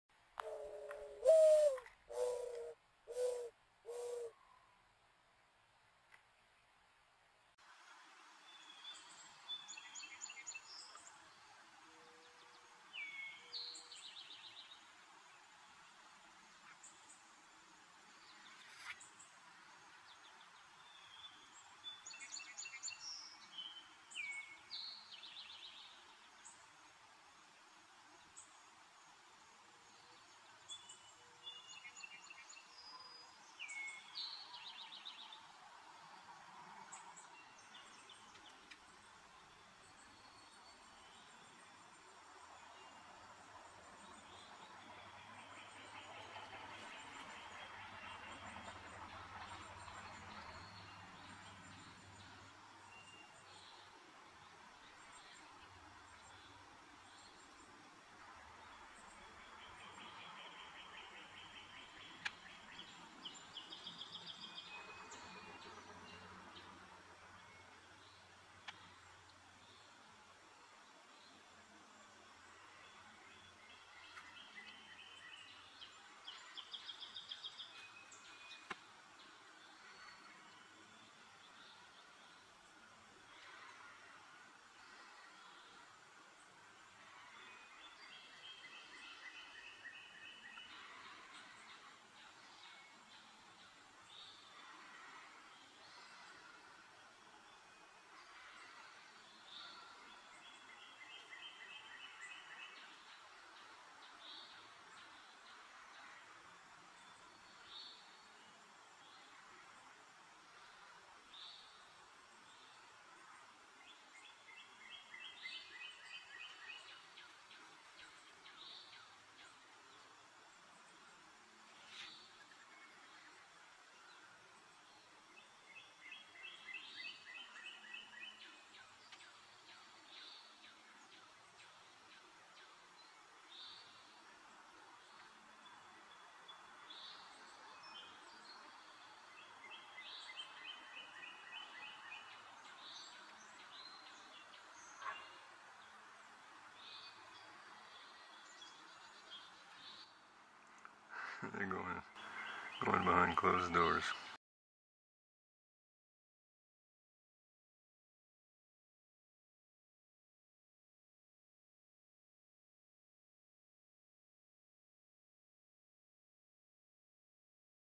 Rabbits "Get a Room"
A slice of Backyard life - Backyard rabbits (Appalachian Cottontails) are known for living their lives out in the open, but these two "lovebirds" take their romance behind the Norway Spruce tree. The 3rd young rabbit thinks it's all about play gets bored and decides to eat some grass instead. Nice Eastern Towhee and Northern Cardinal songs in the background.
Filmed after sunset with the Canon R6 on Amazon
Commission Earned